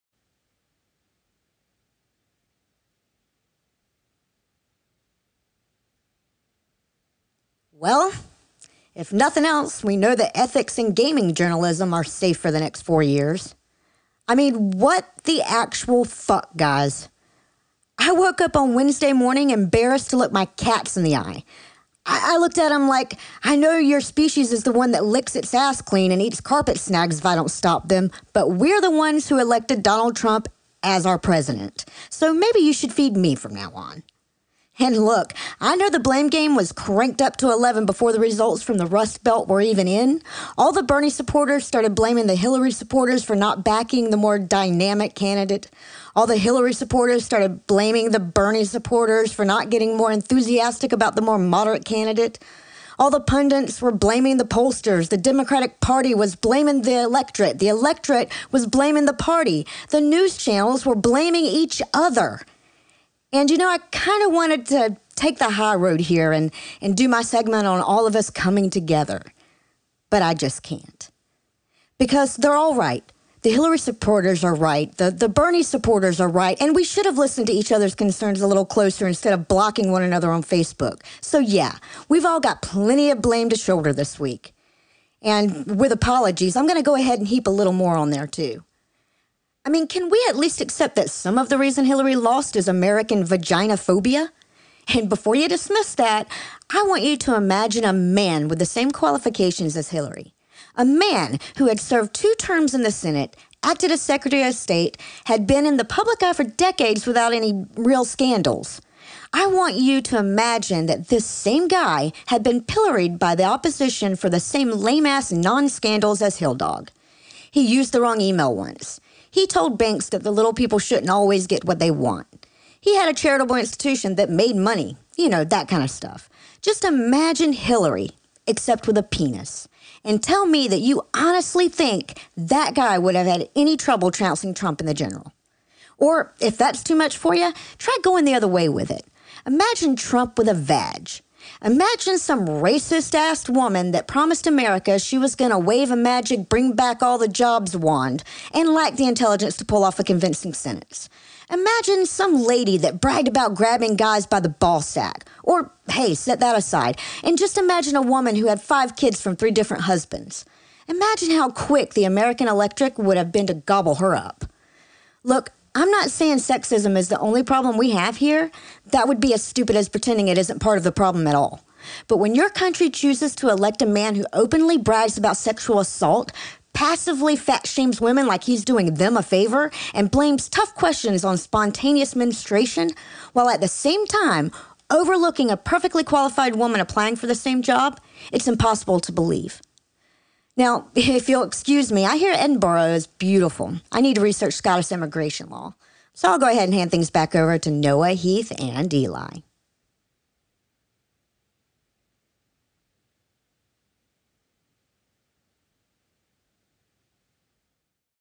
Episode 195 TWIM: If Hillary was a Man
Lucinda lets us know her thoughts on President Donald Trump